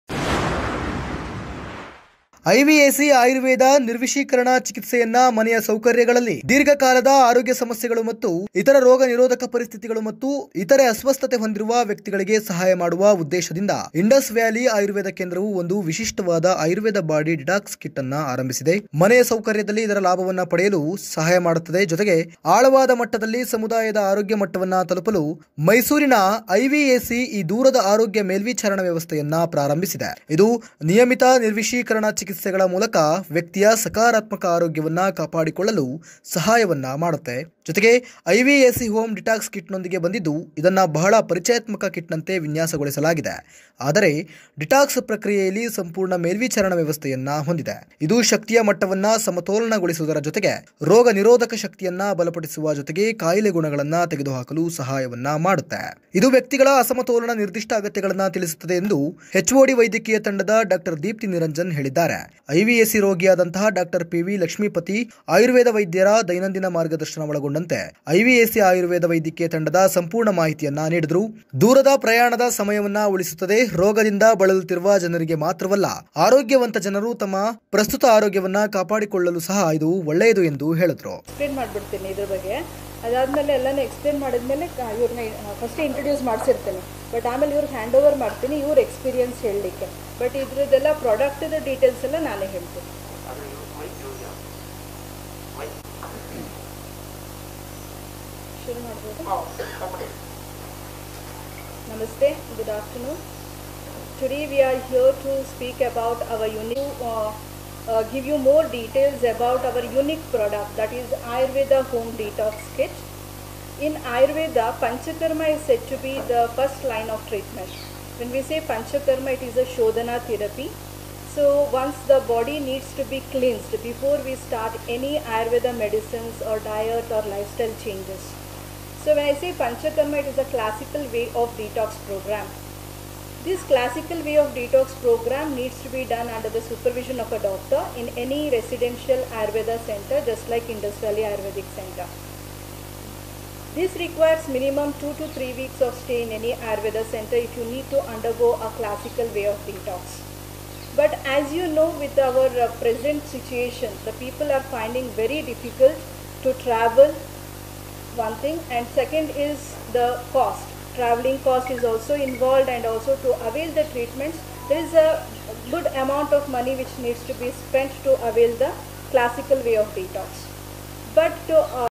Discussing The Benefits Of The Indus Valley Ayurvedic Centre Home Detox Kit
The Indus Valley Medical Team discussing the many benefits of our unique Indus Valley Ayurvedic Centre Home Detox Kit. The Kit was specially developed under the expert guidance of IVAC’s Medical Team and takes place with daily supervision from our medical team, where the patient is monitored closely every day by our dedicated doctors for the duration of the detox program.